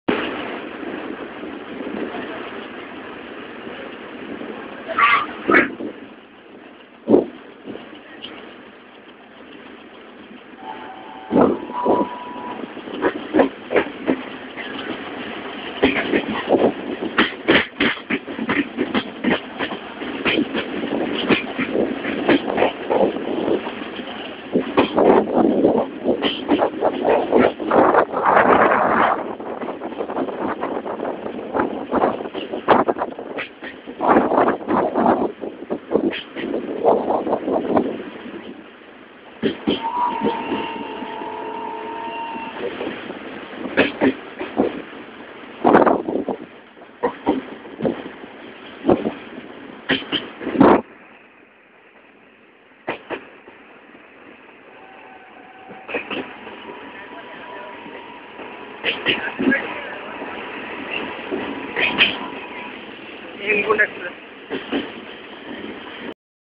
Jammu Tawi-Rishikesh Hemkunt Express at rishikesh track from Raiwala.avi
at rishikesh track raiwala,dehrdaun
leaving raiwala on dehradun express